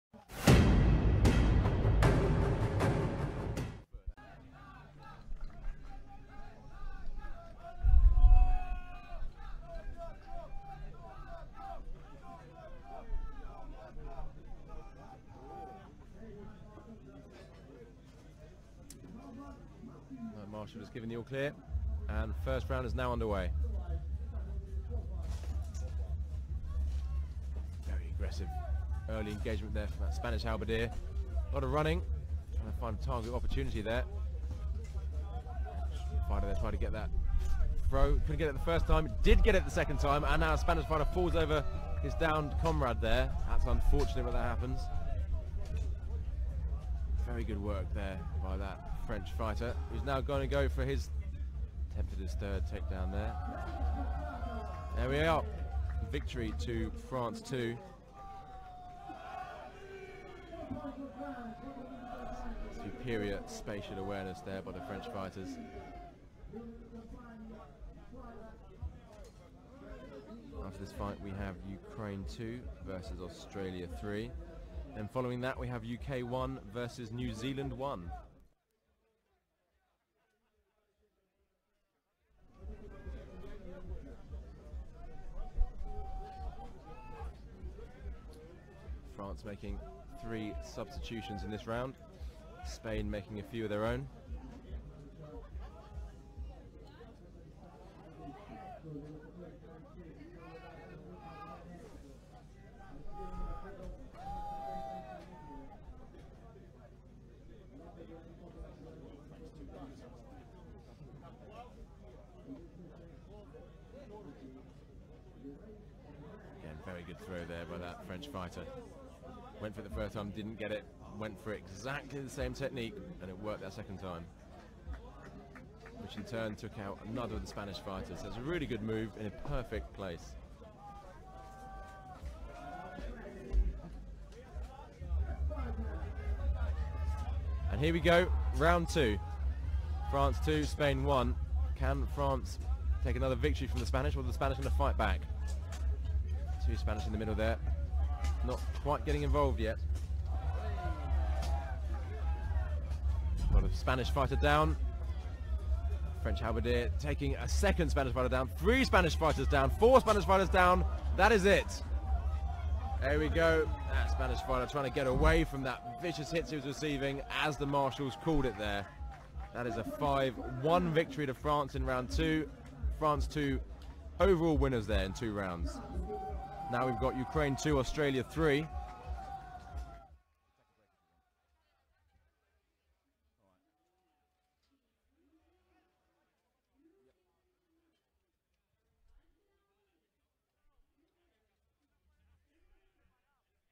Battle of the Nations 2018 - France 2 vs Espagne 1 - 5 vs 5 - Groupe I
France vs Spain

Pour vous équiper et nous rejoindre en lice, nos partenaires équipement Forge of Svan et Medieval Market vous proposent tout ce dont vous avez besoin pour commencer.